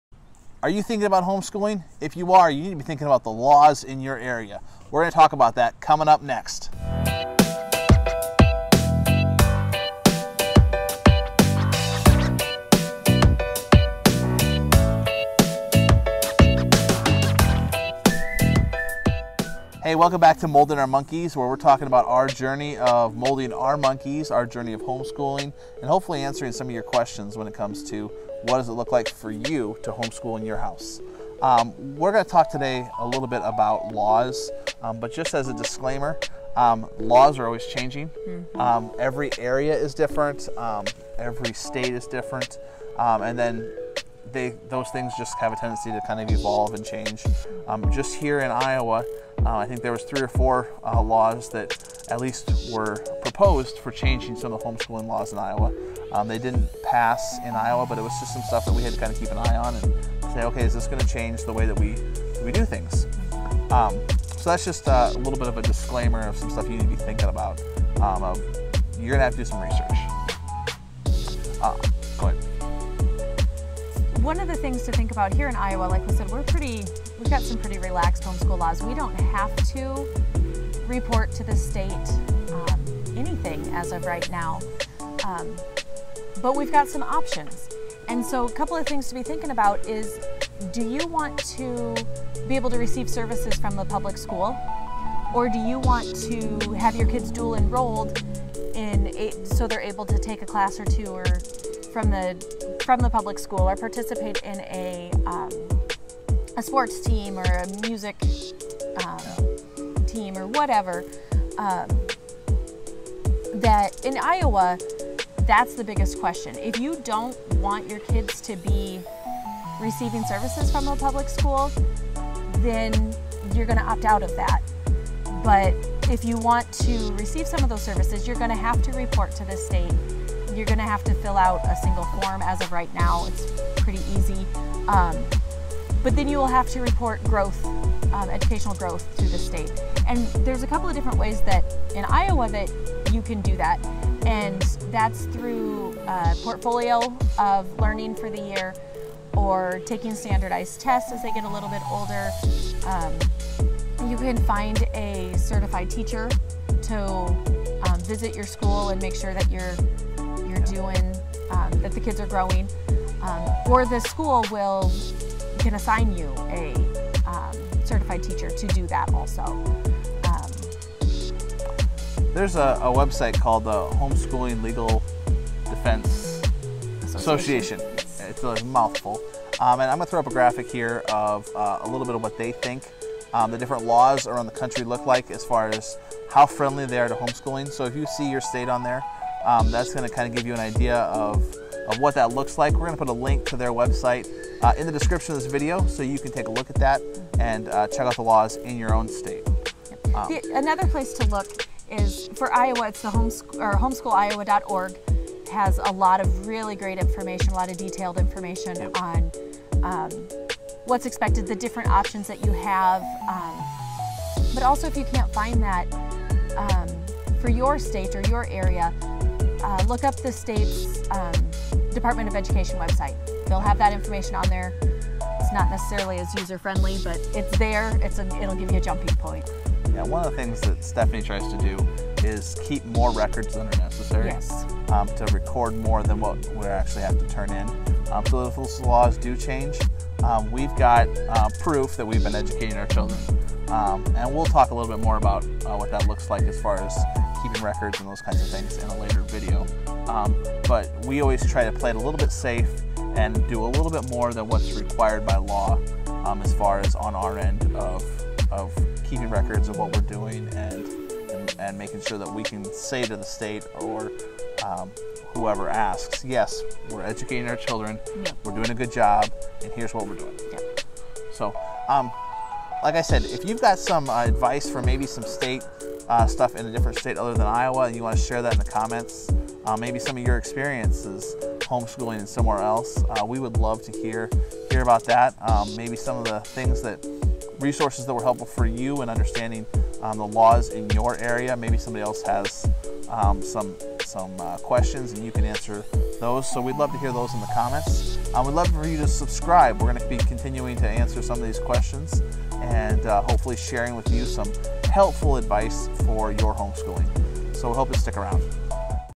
Thinking about homeschooling? (Think about the laws)
If you are thinking about homeschooling, you need to research the laws in your State or country. Laws about homeschooling are often changing, and you will need to understand the laws that affect your homeschooling.

If you were in the United States, a great place to start would be the HSLDA.

Another place to check would be your states education department website.